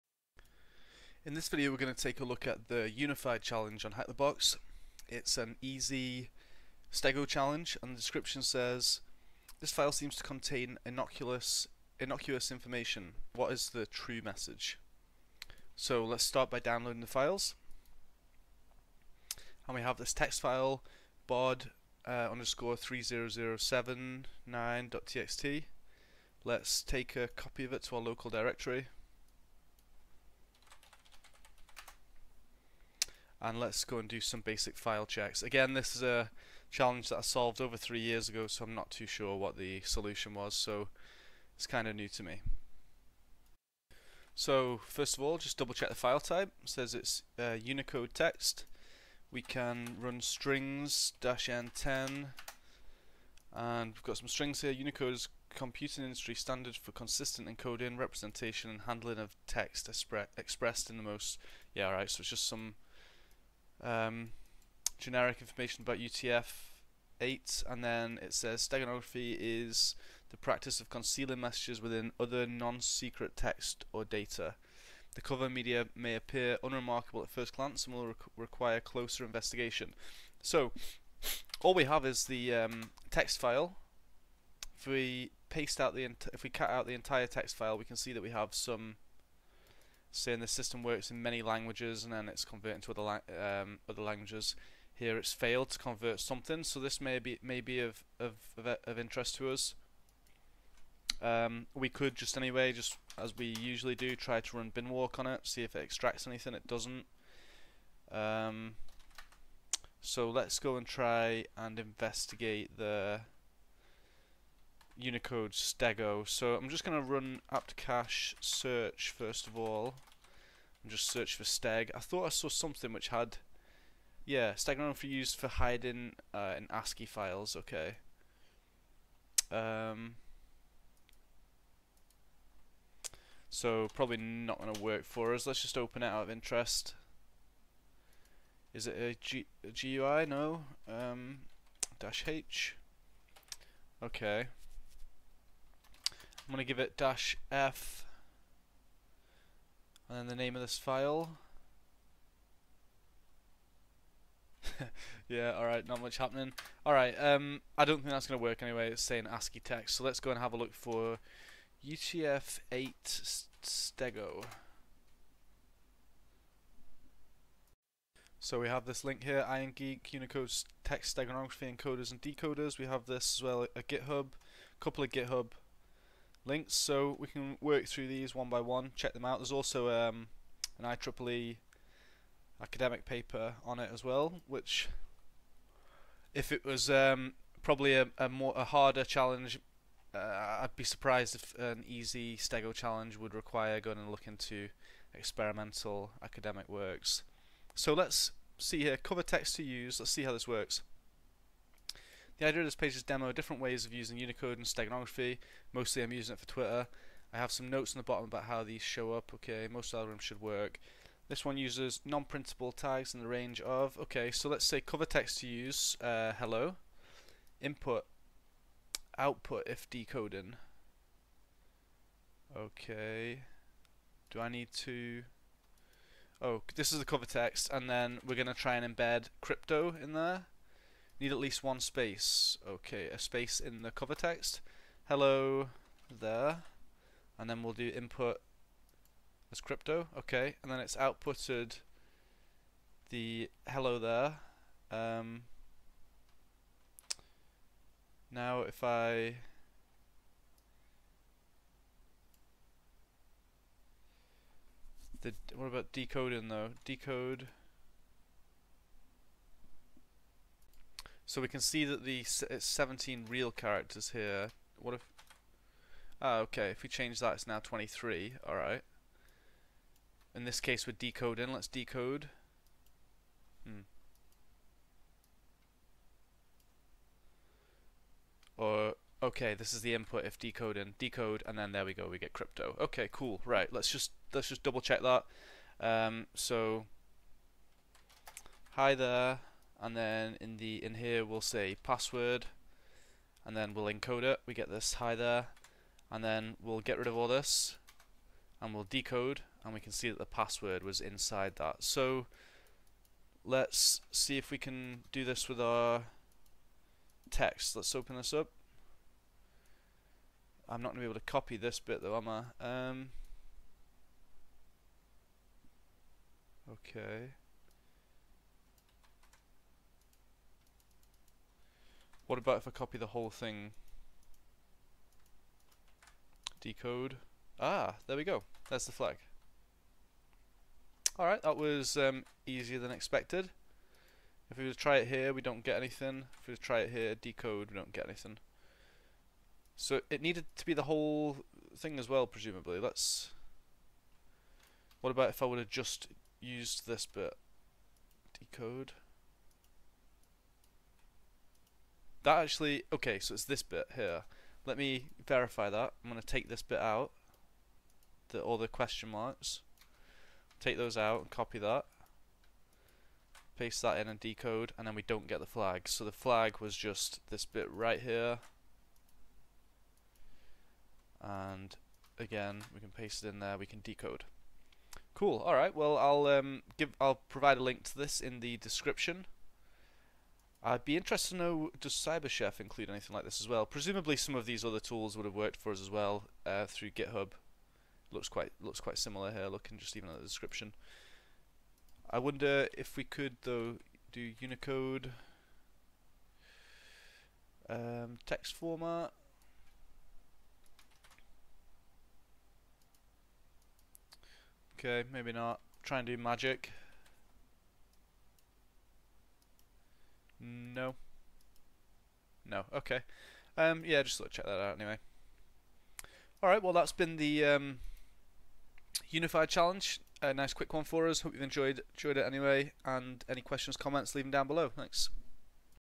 Unified [easy]: HackTheBox Stego Challenge (UTF-8 steganography)
Video walkthrough for retired @HackTheBox (HTB) Stego challenge "Unified" [easy]: "This file seems to contain innocuous information. What is the true message?" - Hope you enjoy 🙂

↢Chapters↣
Start - 0:00
Basic file checks - 0:43
Investigate UTF-8 stego - 2:47
Solve with online decoder - 5:15